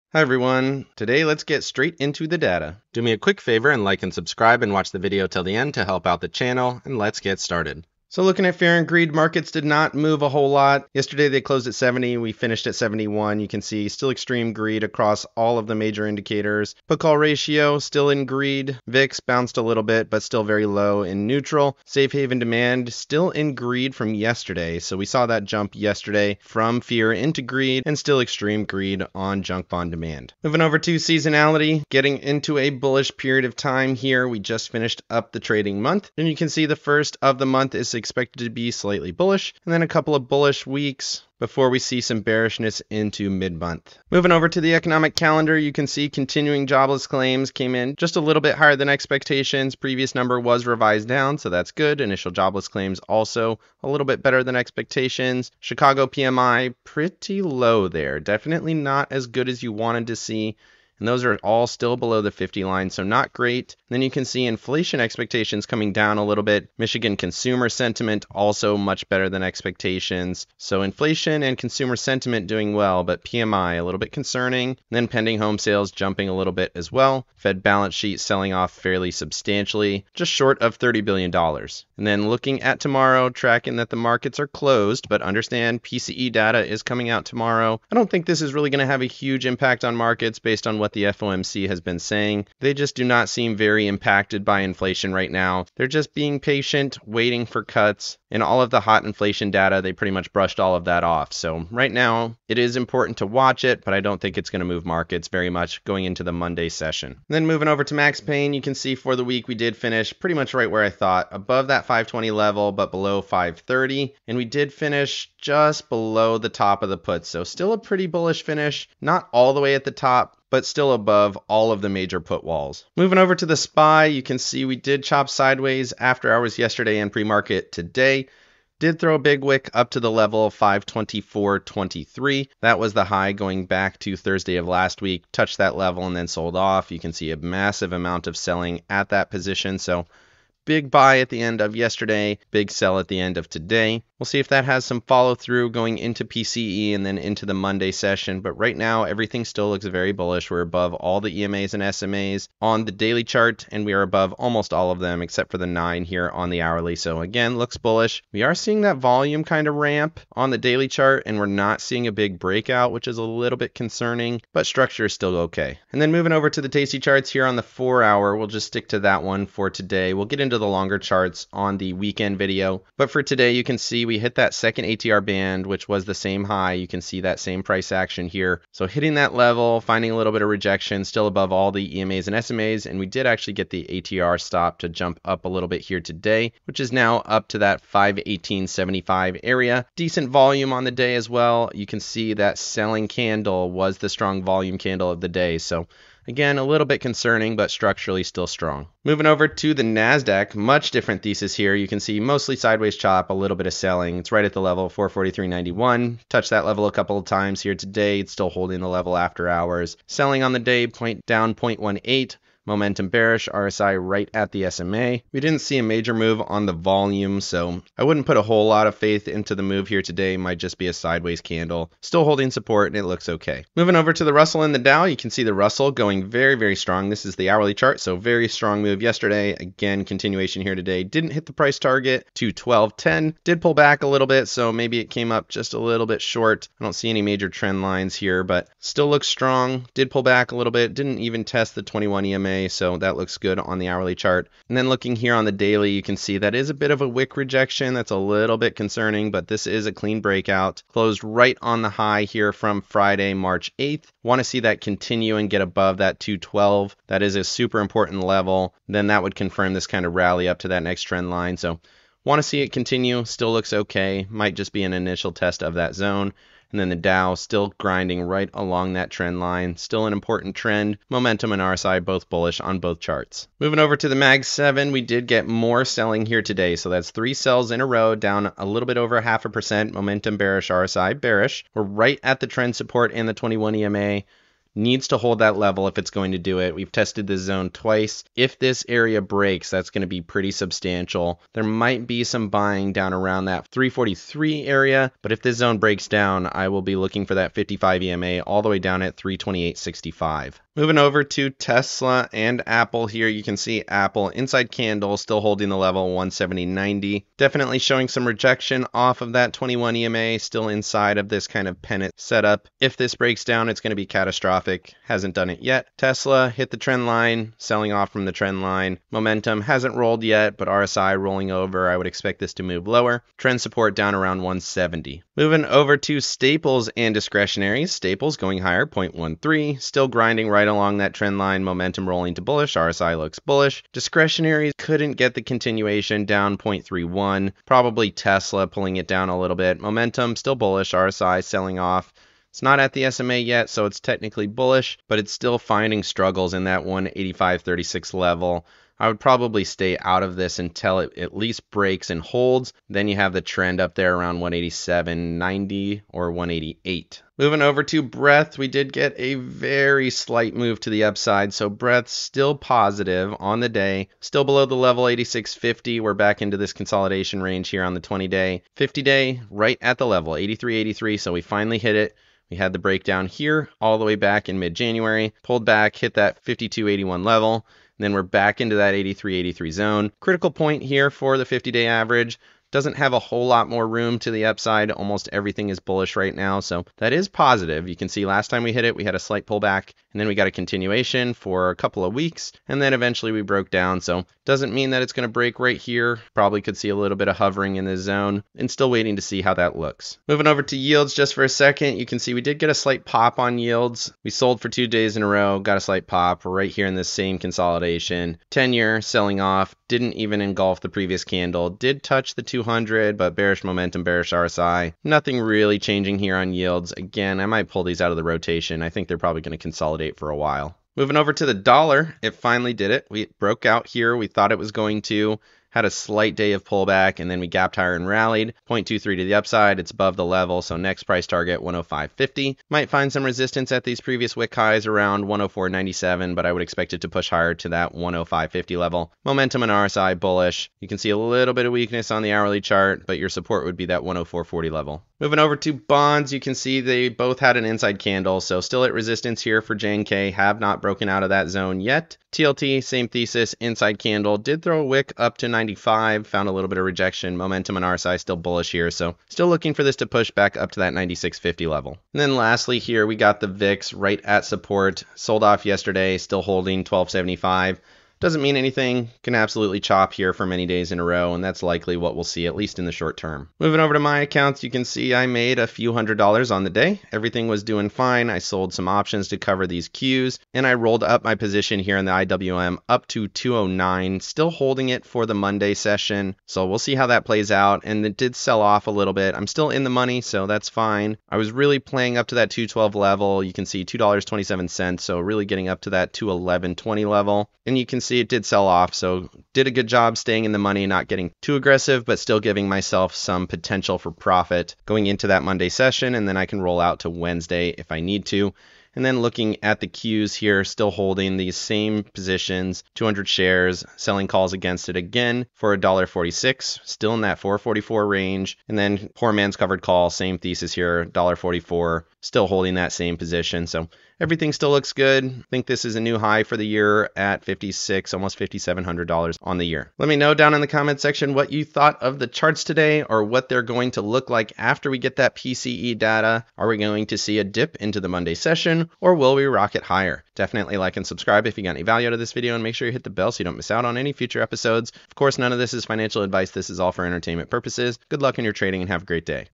Will the Melt-Up Continue? 28MAR Technical Analysis SPY QQQ IWM MAG7 AAPL TSLA XLP XLY TLT VIX
This video goes over seasonality,  the economic calendar, and the charts.

Technical Analysis of SPY QQQ IWM AAPL TSLA XLP XLY TLT VIX.

0:00 Intro
0:08 Fear & Greed
0:30 Seasonality
0:44 Economic Calendar
1:47 Max Pain
2:03 SPY
3:19 QQQ
3:44 IWM DIA
4:33 MAG 7
5:01 AAPL TSLA
5:25 XLP XLY
5:57 % Above 20 50 SMA
6:40 2 10Y Yield
7:01 DXY
7:25 JNK TLT
7:42 VIX
7:53 Portfolio Thoughts
8:50 Outro